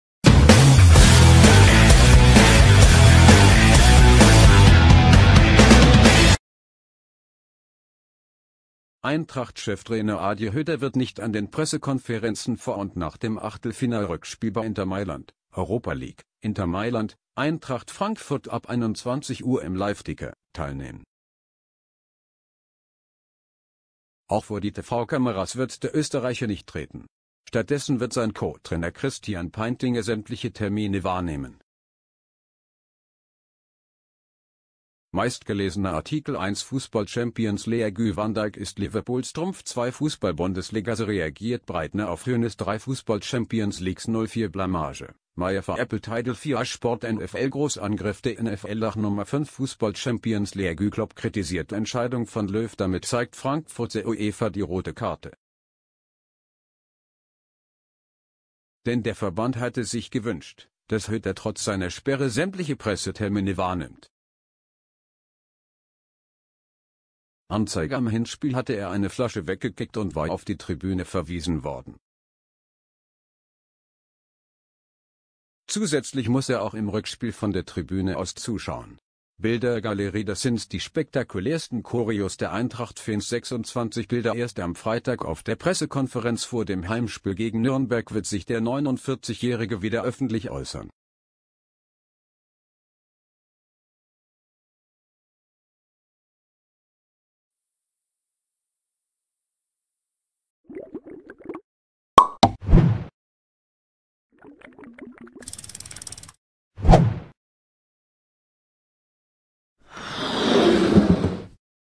Europa League: Eintracht-Trainer Adi Hütter mitPK-Boykott gegen Inter
Eintracht-Cheftrainer Adi Hütter wird nicht an den Pressekonferenzen vor und nach dem Achtelfinal-Rückspiel bei Inter Mailand (Europa League: Inter Mailand - Eintracht Frankfurt ab 21 Uhr im LIVETIC
eintracht frankfurt,fussball,uefa,europa league,eintracht,fußball,uefa europa league